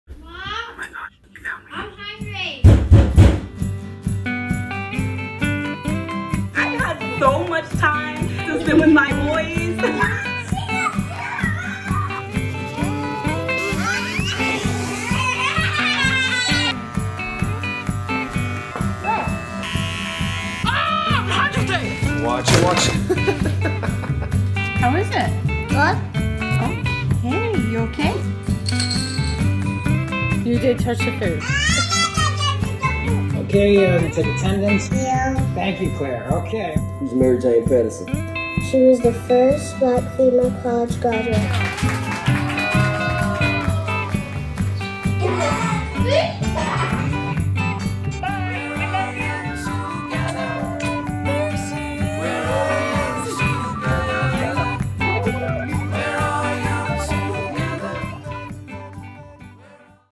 Thank You Parents
Being a parent is different this year because parents have had to become so much more. Thanks to all the moms and dads for being our barbers, teachers, chefs, dance partners, coworkers, and best friends, too.

For helpful resources, check out g.co/thankyouparents